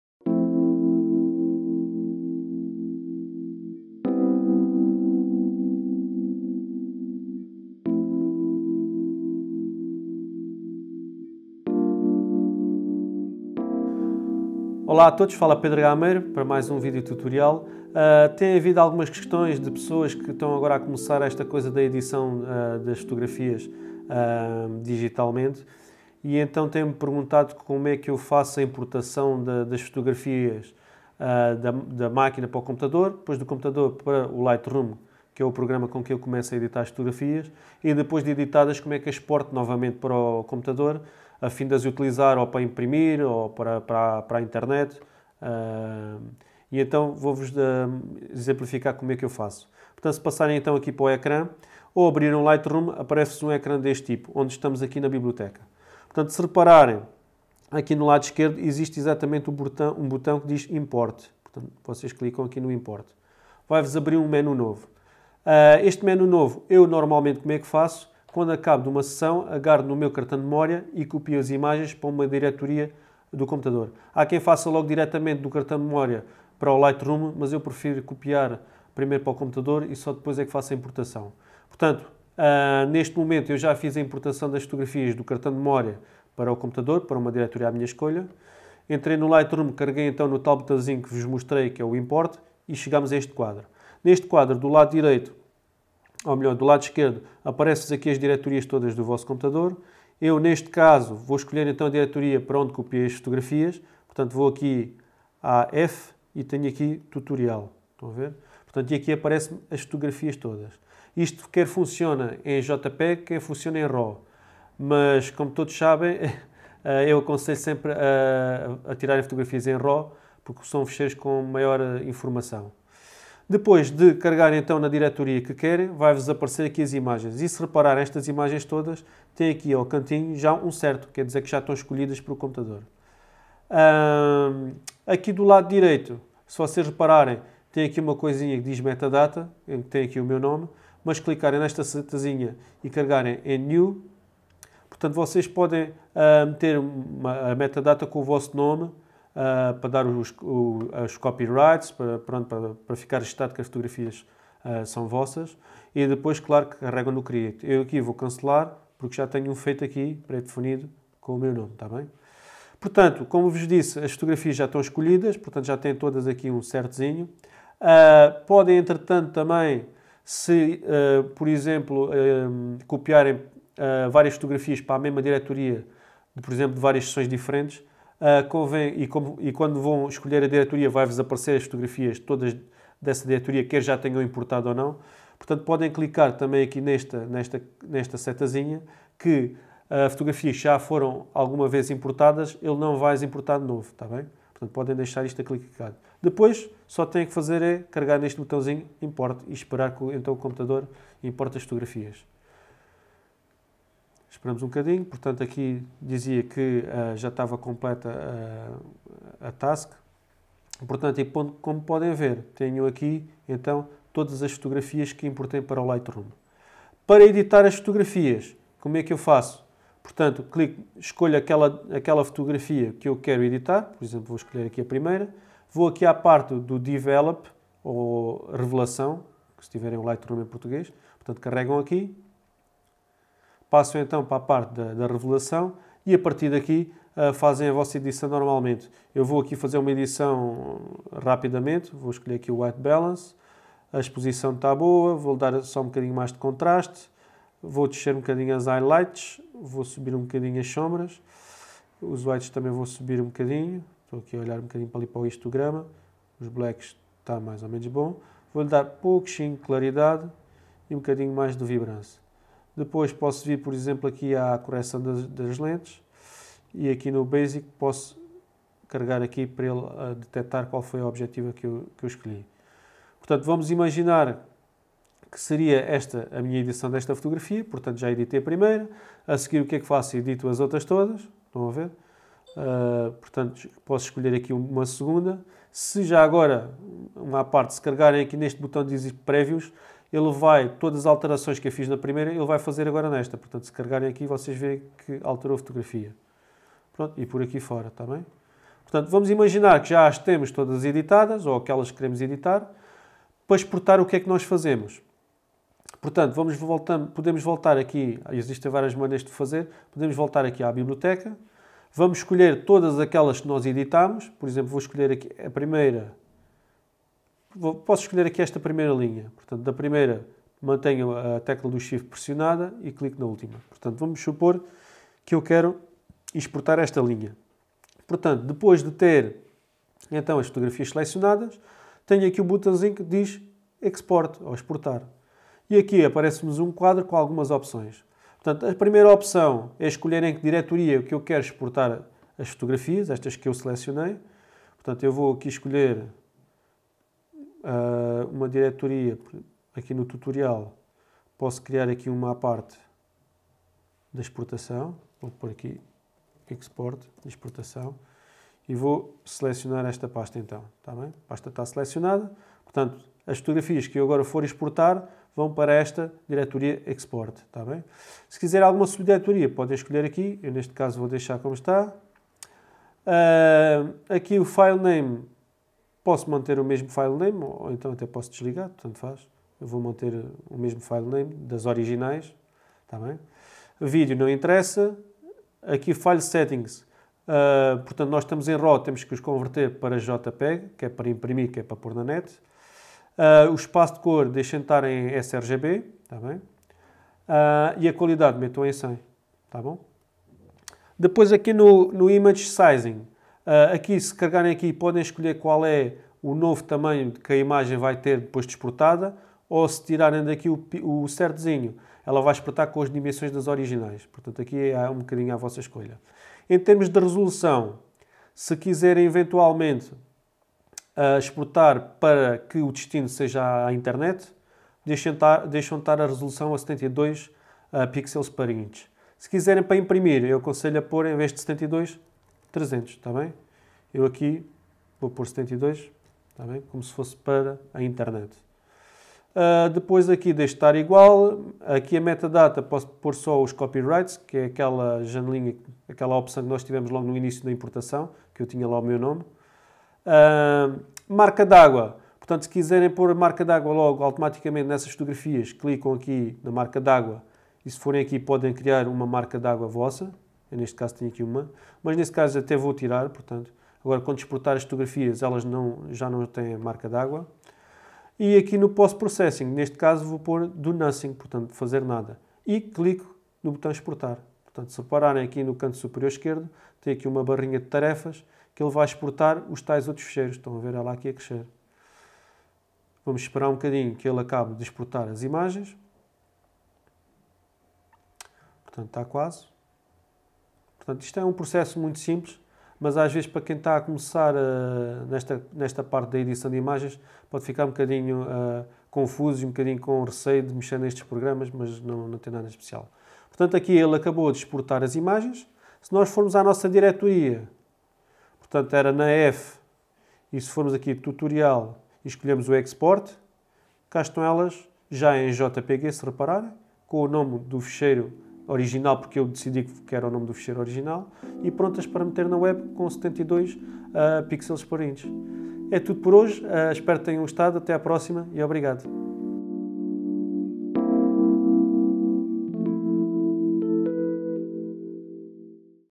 Importar e Exportar no Lightroom
Como importar e exportar as nossas fotografias para o Lightroom